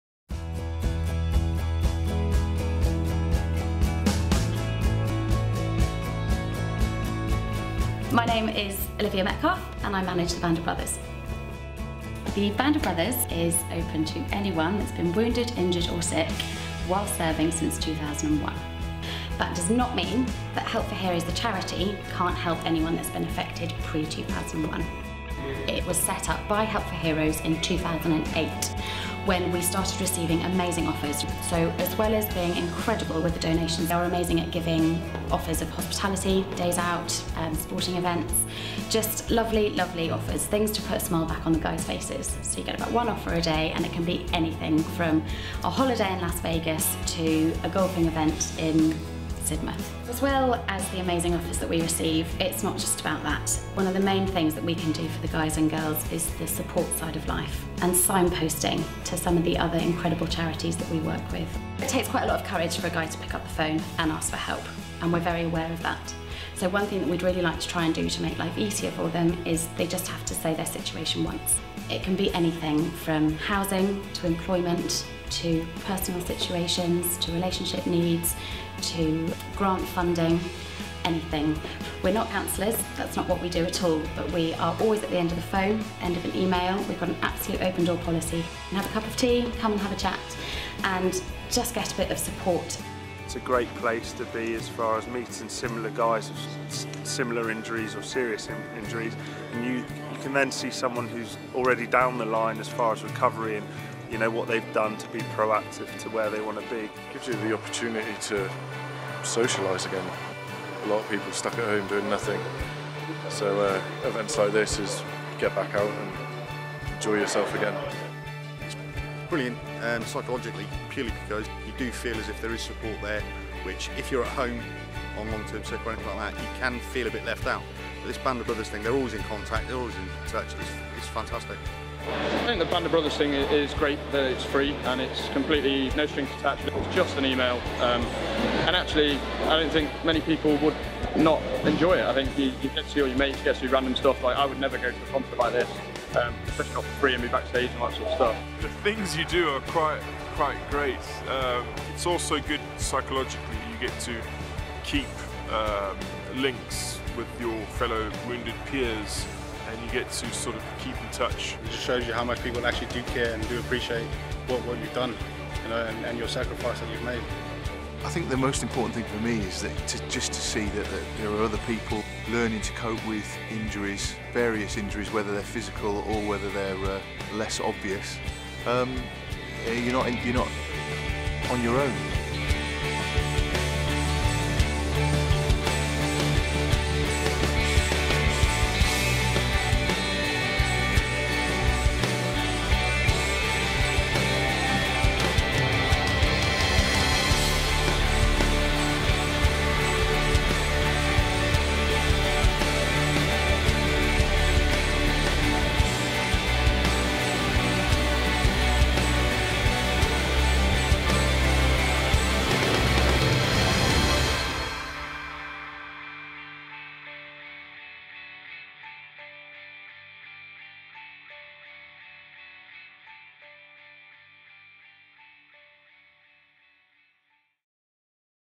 Band of Brothers | Help for Heroes
To join the Band of Brothers contact Olivia Metcalfe via

The Band of Brothers offers life-long support and opportunities to Servicemen or women who have become wounded, injured or sick during, or as a consequence of their service since 9/11.

As a Band of Brothers member, you will receive offers of hospitality, employment, holidays and so much more which are donated to us by the very supportive public. We enable our members to participate in a wide range of new and exciting activities in a fun and supportive environment.

In addition to distributing these exciting offers, we also signpost our members to civilian and military support and welfare organisations when needed.